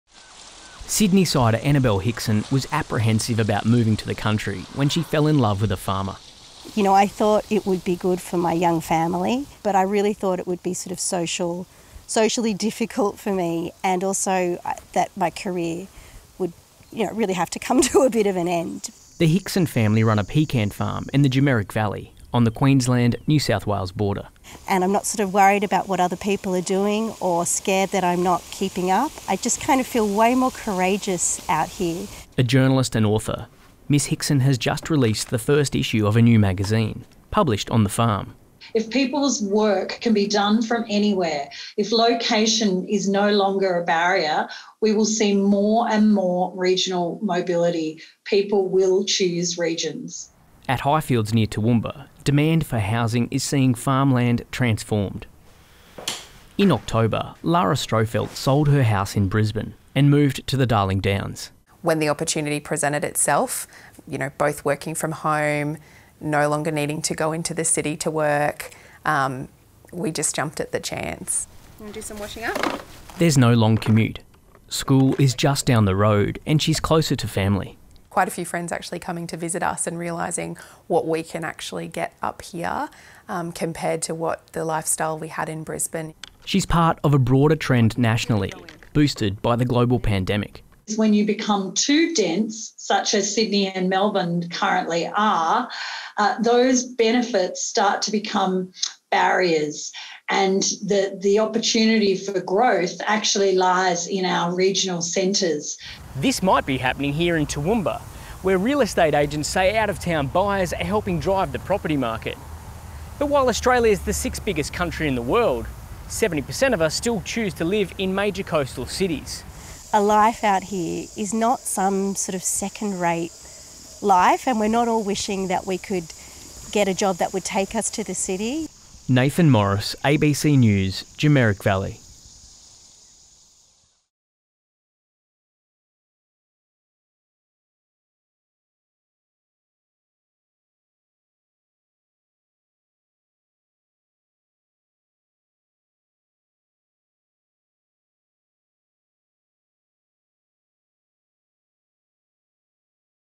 COVID-19 helping drive regional growth as flexible work sees people look beyond the city | ABC News
The COVID-19 pandemic is helping to drive growth in regional Australia, with flexible work making it possible for people to relocate beyond built up-coastal cities, according to a peak regional research body.